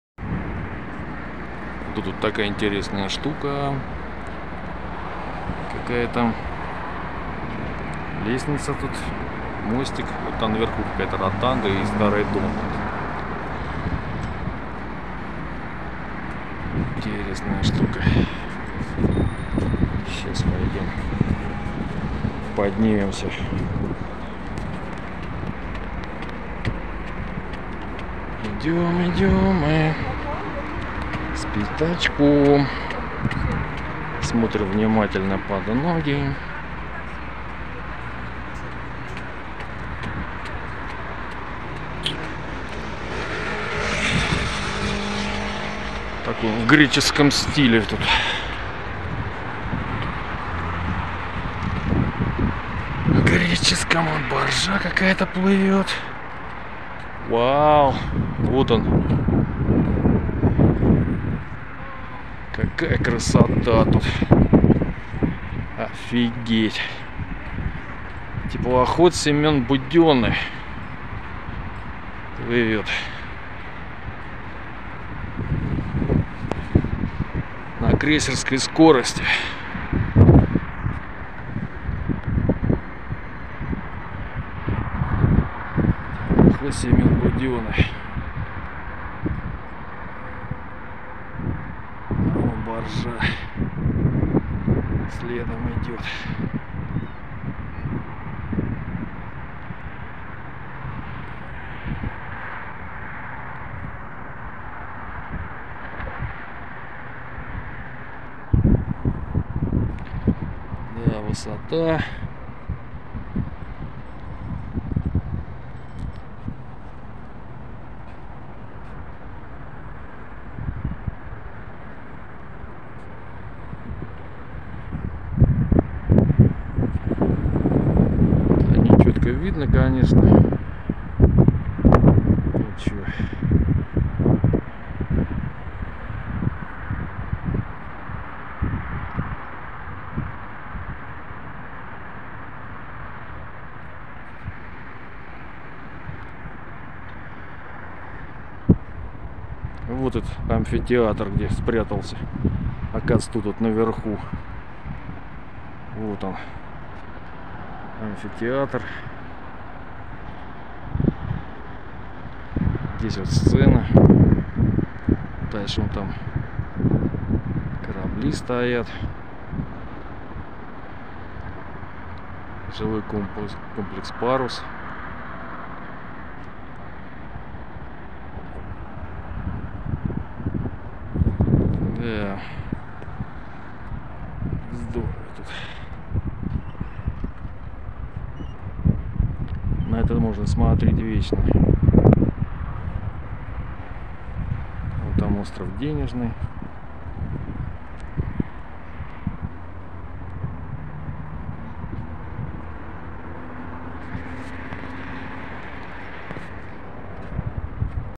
Волгоградский Амфитеатр на Волге
Поехали прогуляться на Набережную в Волгограде и случайно обнаружили Амфитеатр, который построили недавно.
Получилось красиво. Но ещё красивее вид на Волгу.